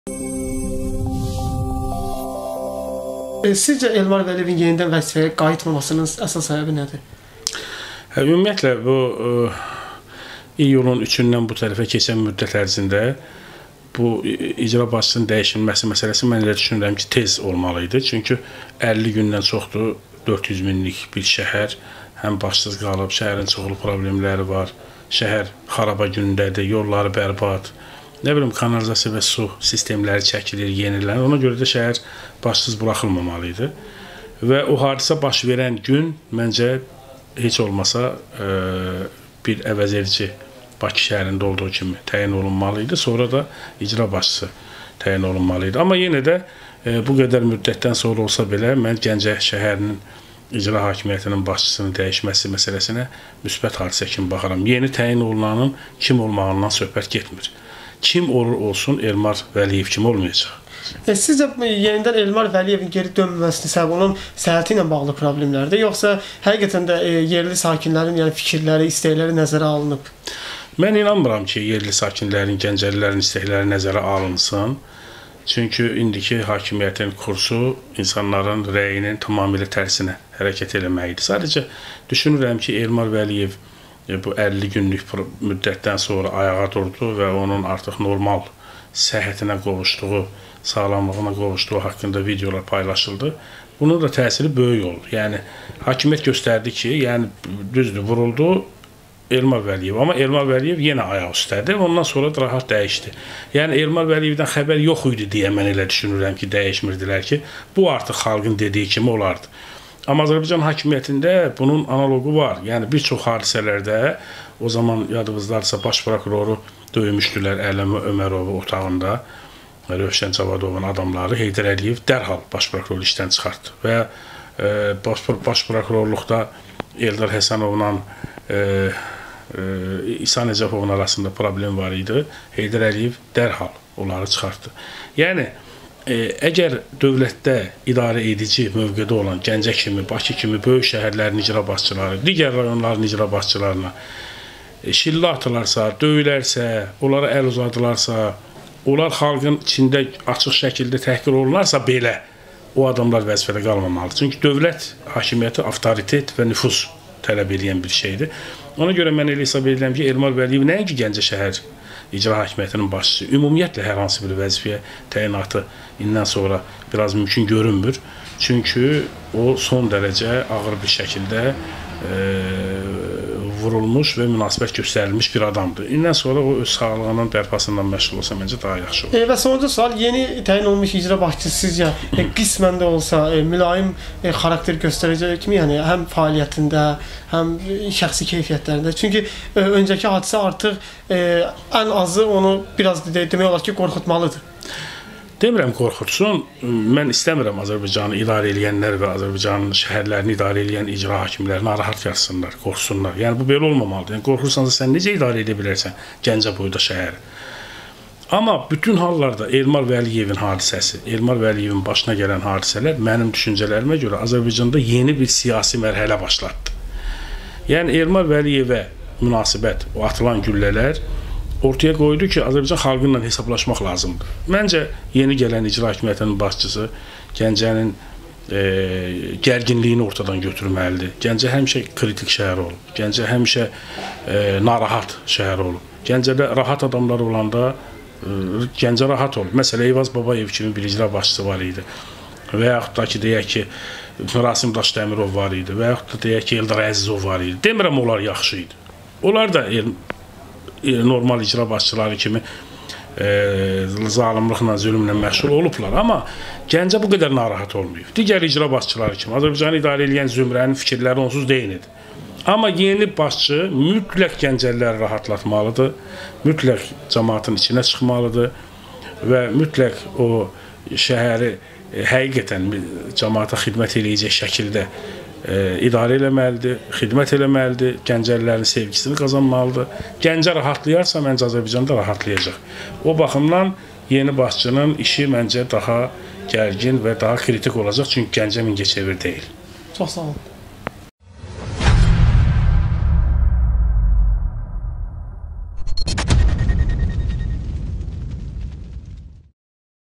"Elmar Vəliyevə atılan güllə onu göstərdi ki, xalqla hesablaşmaq lazımdır"- Əvəz Zeynallı
"Xural" qəzetinin baş redaktoru Əvəz Zeynallı kanal 13-ə müsahibə verib. Əvəz Zeynallı Gəncə şəhərinin icra başçısı Elmar Vəliyevin vəzifəsindən kənralaşdırılması məsələsinə münasibət bildirərək deyib ki, icra başçısı gələn istənilən şəxs Elmar Vəliyevdən daha yaxşı olacaq.